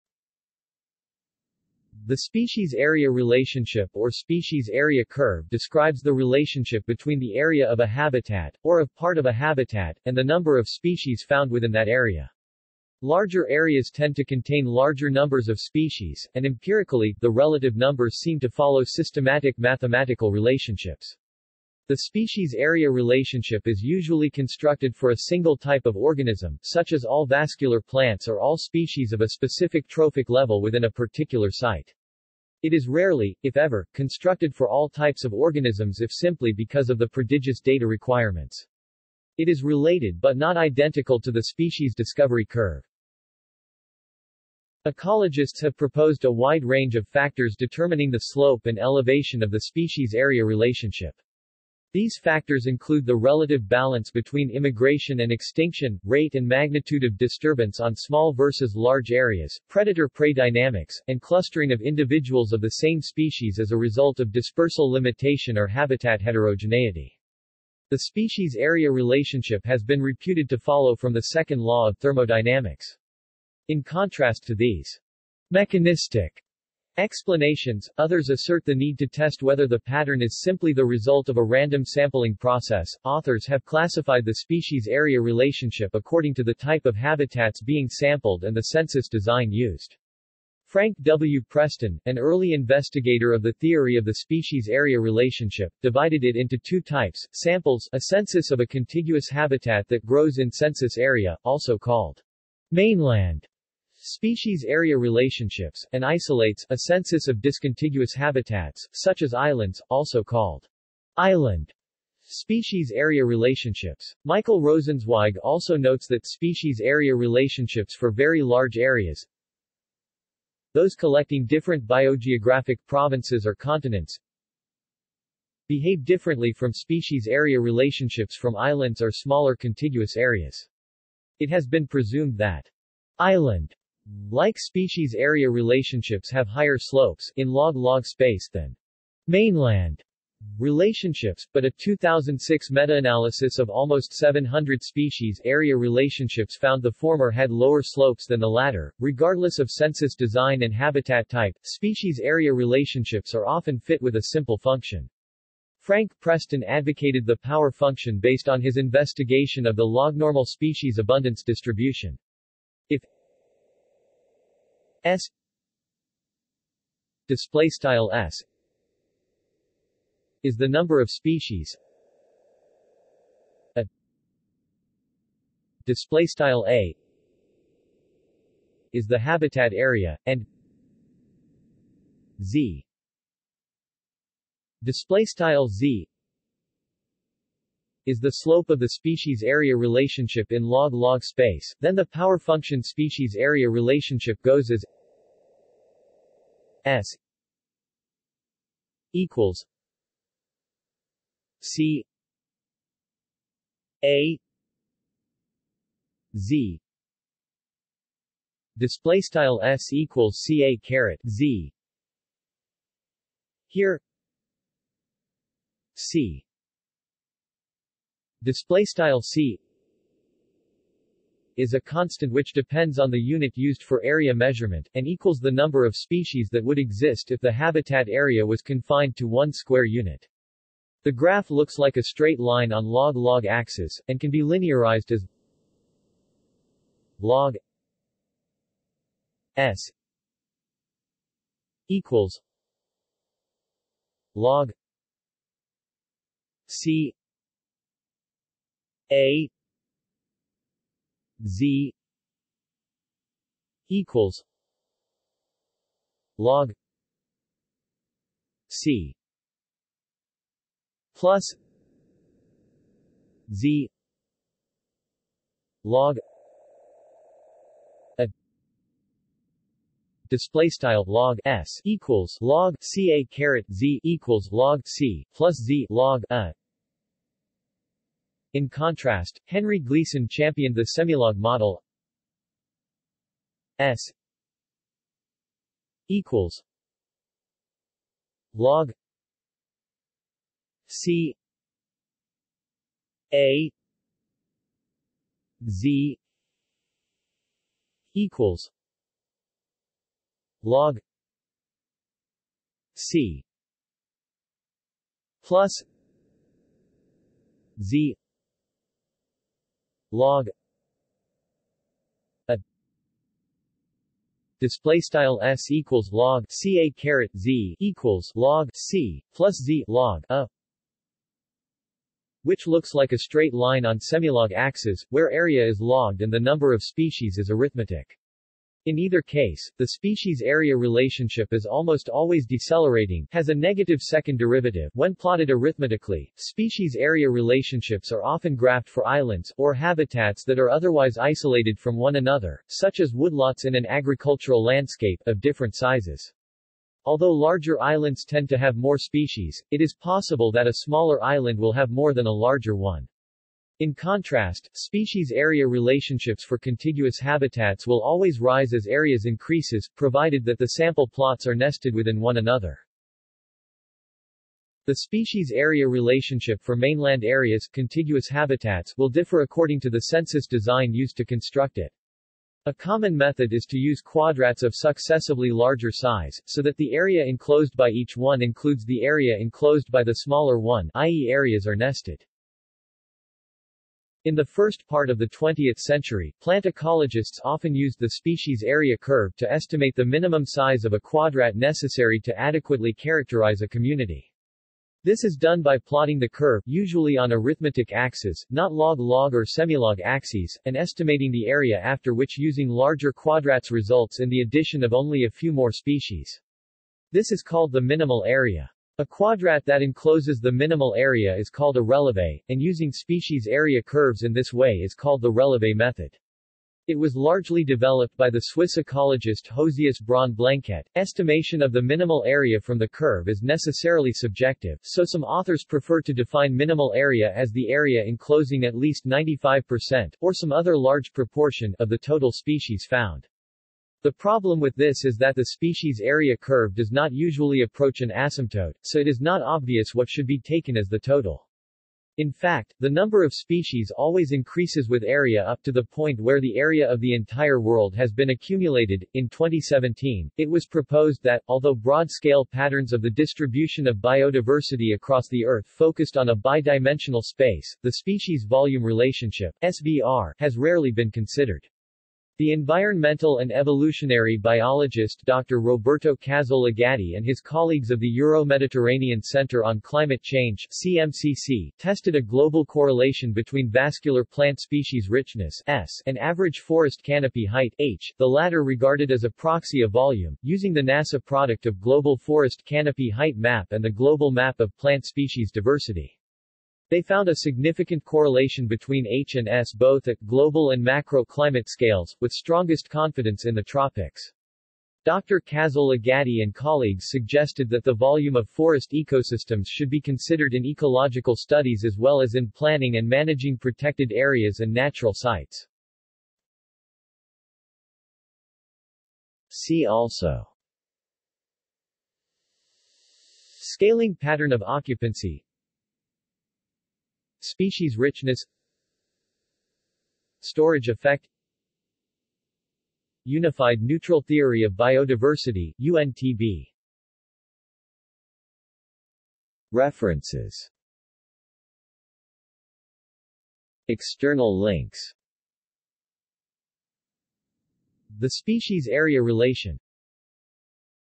Species-area curve | Wikipedia audio article
This is an audio version of the Wikipedia Article:
Species-area curve

SUMMARY
The species–area relationship or species–area curve describes the relationship between the area of a habitat, or of part of a habitat, and the number of species found within that area. Larger areas tend to contain larger numbers of species, and empirically, the relative numbers seem to follow systematic mathematical relationships. The species–area relationship is usually constructed for a single type of organism, such as all vascular plants or all species of a specific trophic level within a particular site. It is rarely, if ever, constructed for all types of organisms if simply because of the prodigious data requirements. It is related but not identical to the species discovery curve.
Ecologists have proposed a wide range of factors determining the slope and elevation of the species–area relationship. These factors include the relative balance between immigration and extinction, rate and magnitude of disturbance on small vs. large areas, predator–prey dynamics, and clustering of individuals of the same species as a result of dispersal limitation or habitat heterogeneity. The species–area relationship has been reputed to follow from the 2nd law of thermodynamics. In contrast to these "mechanistic" explanations, others assert the need to test whether the pattern is simply the result of a random sampling process.Authors have classified the species–area relationship according to the type of habitats being sampled and the census design used. Frank W. Preston, an early investigator of the theory of the species–area relationship, divided it into two types: samples (a census of a contiguous habitat that grows in census area, also called "mainland" species–area relationships), and isolates (a census of discontiguous habitats, such as islands, also called "island" species–area relationships). Michael Rosenzweig also notes that species–area relationships for very large areas—those collecting different biogeographic provinces or continents—behave differently from species–area relationships from islands or smaller contiguous areas. It has been presumed that "island"-like species–area relationships have higher slopes (in log–log space) than "mainland" relationships, but a 2006 metaanalysis of almost 700 species–area relationships found the former had lower slopes than the latter.Regardless of census design and habitat type, species–area relationships are often fit with a simple function. Frank Preston advocated the power function based on his investigation of the lognormal species-abundance distribution. If

        S

    {\displaystyle S}
   is the number of species,

        A

    {\displaystyle A}
   is the habitat area, and

        z

    {\displaystyle z}
   is the slope of the species area relationship in log-log space, then the power function species–area relationship goes as:

        S
        c

          A

            z

    {\displaystyle S=cA^{z}}

Here

        c

    {\displaystyle c}
   is a constant which depends on the unit used for area measurement, and equals the number of species that would exist if the habitat area was confined to one square unit. The graph looks like a straight line on log–log axes, and can be linearized as:

        log
        S
        log
        c

          A

            z

        log
        c
        z
        log

    {\displaystyle \log(S)=\log(cA^{z})=\log(c)+z\log(A)}

In contrast, Henry Gleason championed the semilog model:

        S
        log
        c

          A

            z